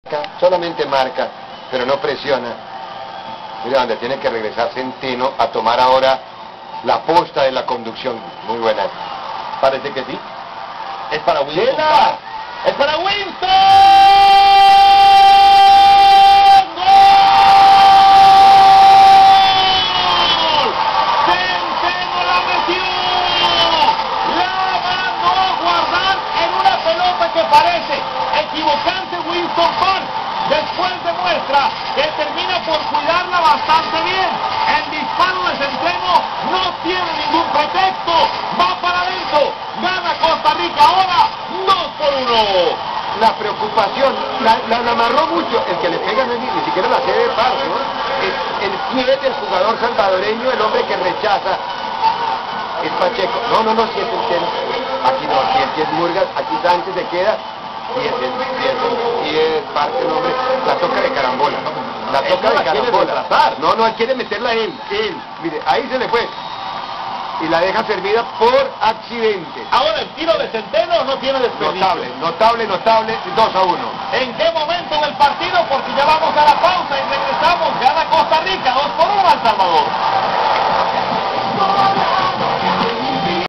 Costa Rica vs El Salvador (2-1) [Copa Oro 2003]
Gol de Walter "Pate" Centeno contra El Salvador para poner el 2-1